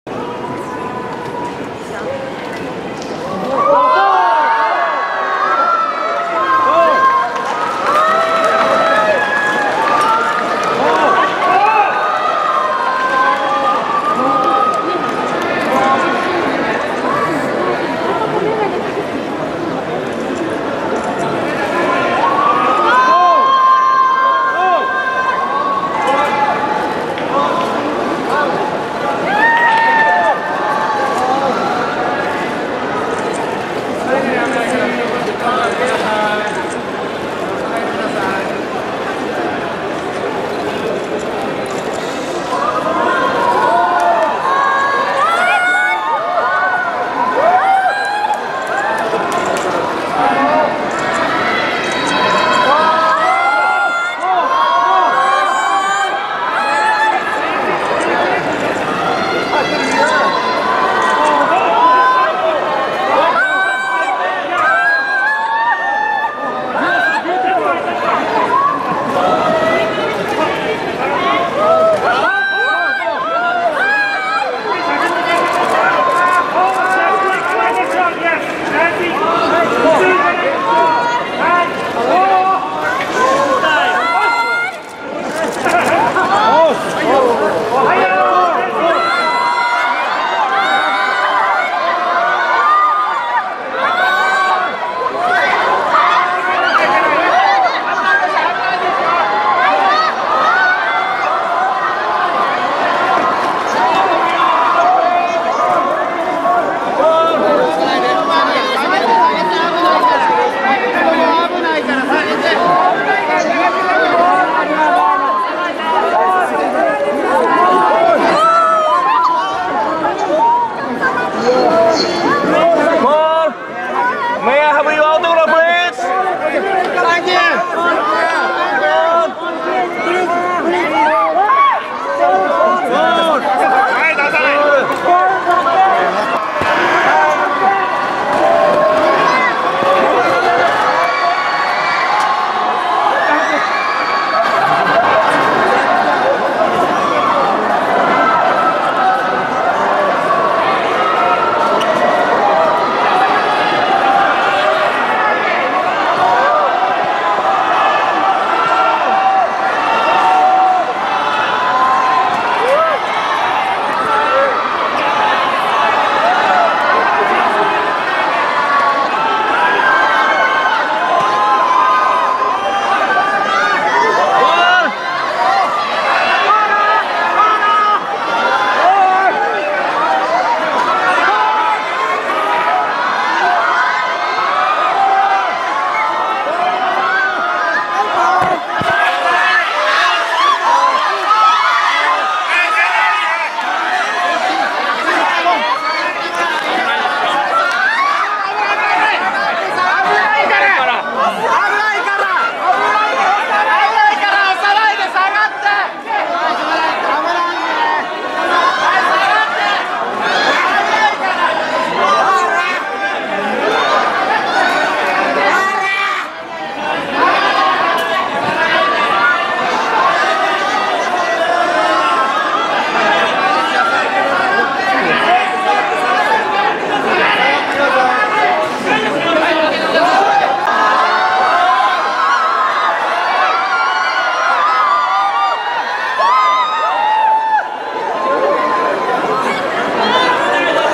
ポール・マッカートニー来日で大阪,関空が大パニック 20150.4.20 Paul Mccartney in Japan Osaka Airport Panic
２１日の京セラドーム公演からはじまる日本ツアーのために来日したポール・マッカートニーを関西国際空港でお出迎え。早朝にもかかわらず大勢のファンが迎えました。